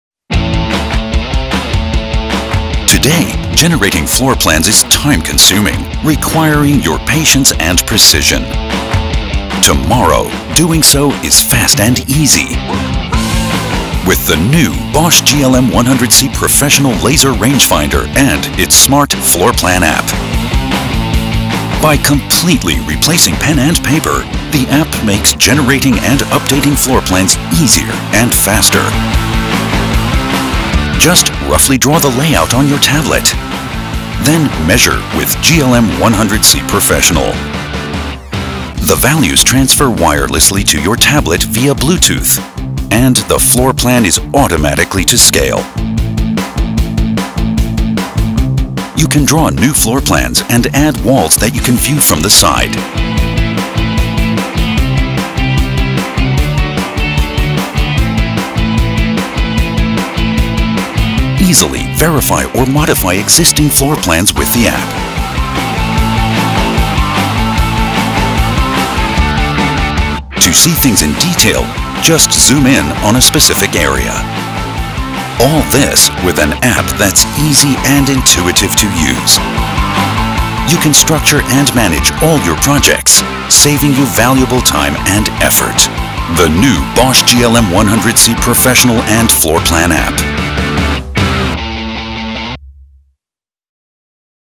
Bosch Professional GLM floor plan App
GLM floor plan:
Optimum workflow thanks to digital floor plans
Simply connect and instantly measure. Connect your tablet PC to the GLM 100 C Professional to perform the first measurements instantly.